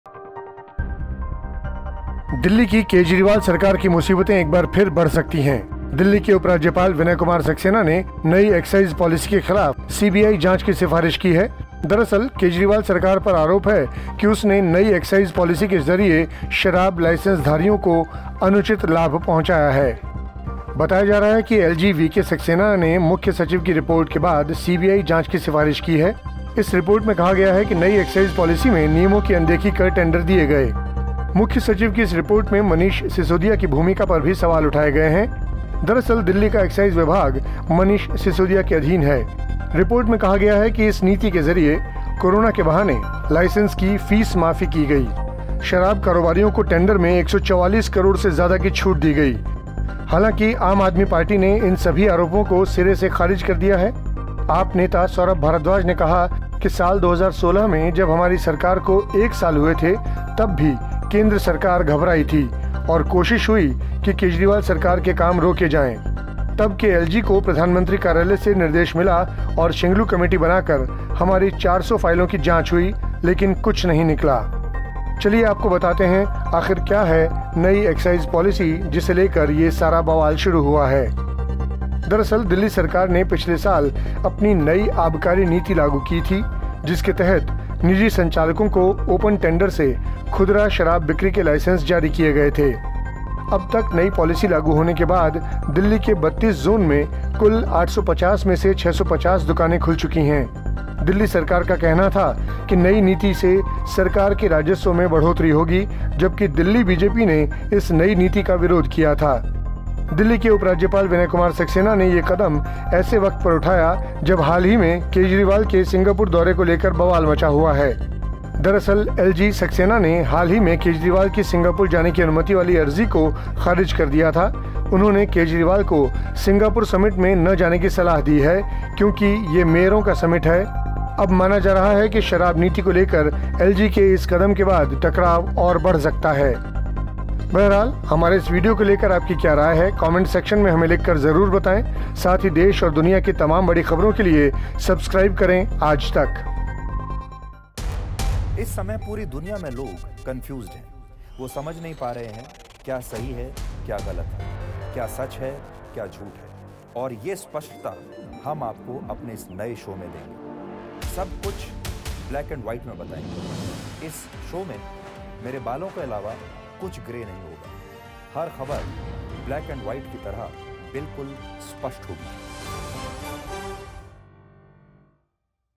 Delhi में LG और CM Kejriwal में दो-दो हाथ, Excise Policy की होगी CBI जांच | Arvind Kejriwal Vs LG
Delhi Excise Policy: दिल्ली की केजरीवाल सरकार की मुसीबतें एक बार फिर बढ़ सकती हैं. दिल्ली के उपराज्यपाल विनय कुमार सक्सेना ने नई एक्साइज पॉलिसी के खिलाफ सीबीआई जांच की सिफारिश की है. दरअसल केजरीवाल सरकार पर आरोप है कि उसने नई एक्साइज पॉलिसी के जरिए शराब लाइसेंसधारियों को अनुचित लाभ पहुंचाया है. बताया जा रहा है कि एलजी वीके सक्सेना ने मुख्य सचिव की रिपोर्ट के बाद सीबीआई जांच की सिफारिश की है. इस रिपोर्ट में कहा गया है कि नई एक्साइज पॉलिसी में नियमों की अनदेखी कर टेंडर दिए गए.

The troubles of the Kejriwal government of Delhi may increase once again. Delhi Lieutenant Governor Vinay Kumar Saxena has recommended a CBI inquiry against the new excise policy. In fact, the Kejriwal government is accused of giving undue benefits to the liquor licensees through the new excise policy. It is being told that LG VK Saxena has recommended a CBI inquiry after the Chief Secretary's report. It has been said in this report that tenders were given ignoring the rules in the new excise policy.